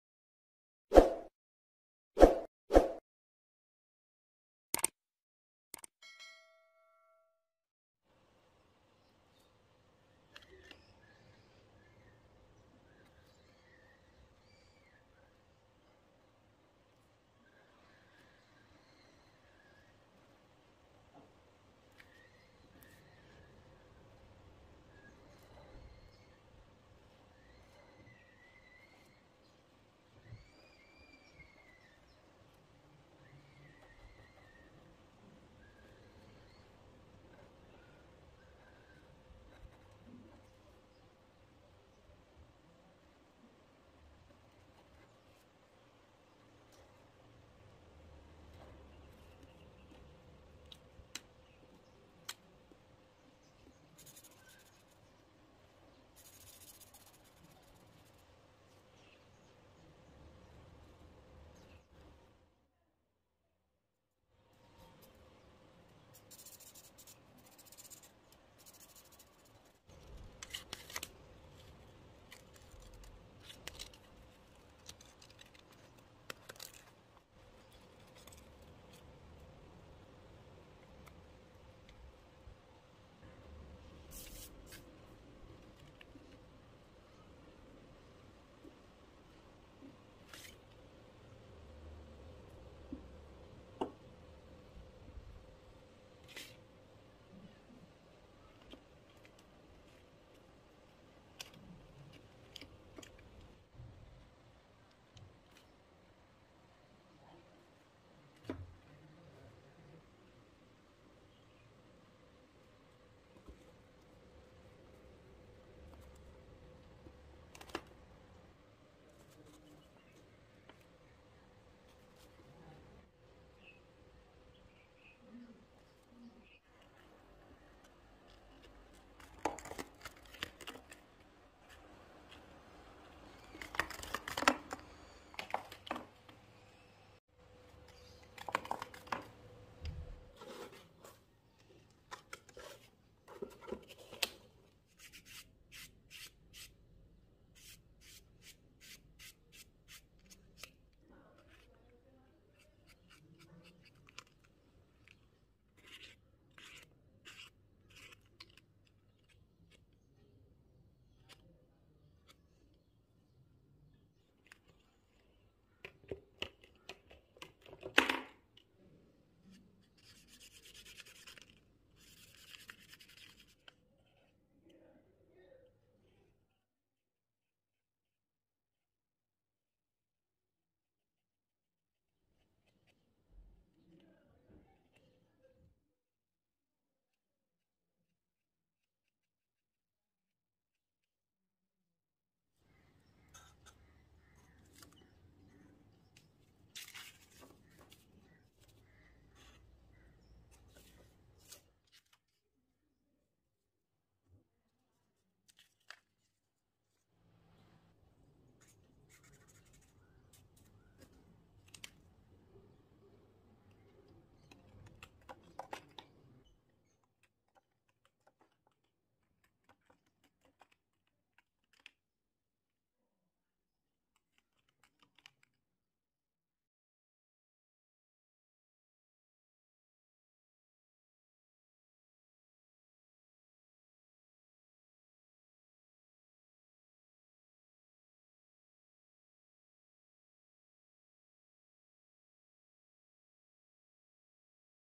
Tumbling Toy - 3 different projects | viewer's request
In this video you will learn how to display your tumbling toy as a project in 3 different ways

how to make a rolling toy.